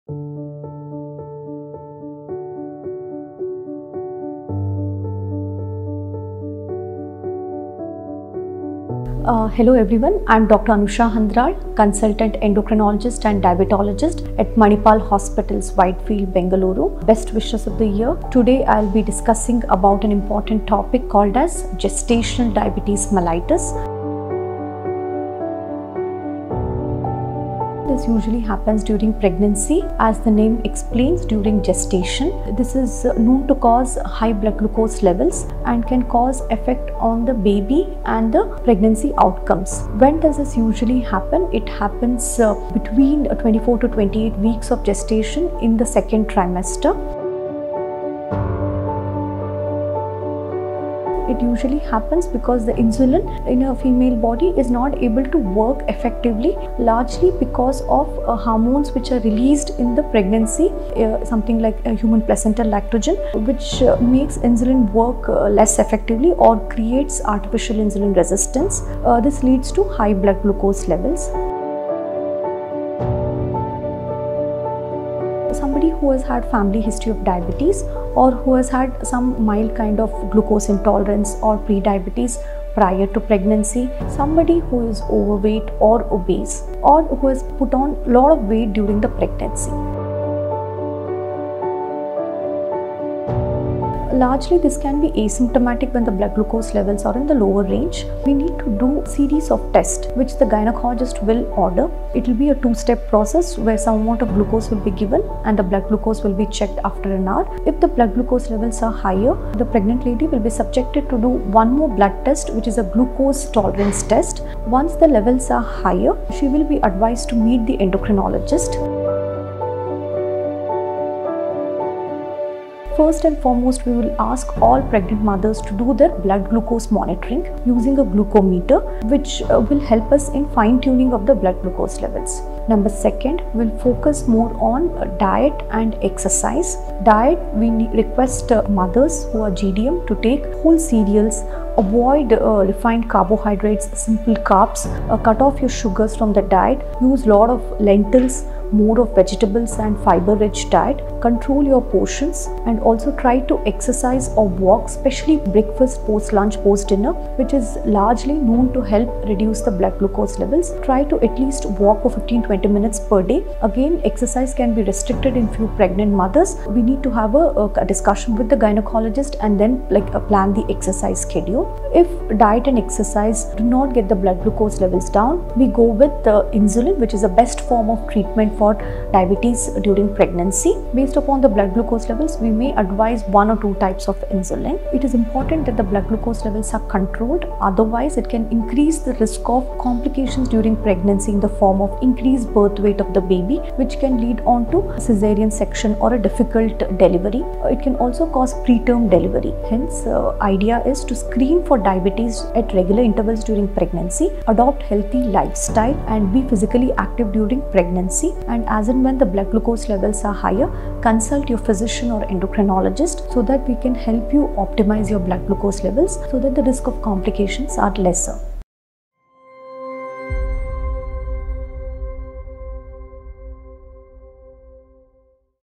Gestational Diabetes Mellitus | Dr. Anusha Handral | Manipal Hospital Whitefield
Dr. Anusha Handral, Consultant - Diabetes and Endocrinology, Manipal Hospital Whitefield, discusses gestational diabetes mellitus (GDM). She speaks about the causes and risk factors associated with this condition, and highlights the importance of monitoring and controlling blood sugar levels. Watch the video to know more.

This video is for awareness only and should not be considered for the purpose of self-diagnosis or medication. Seek a qualified clinical practitioner for diagnosis and treatments.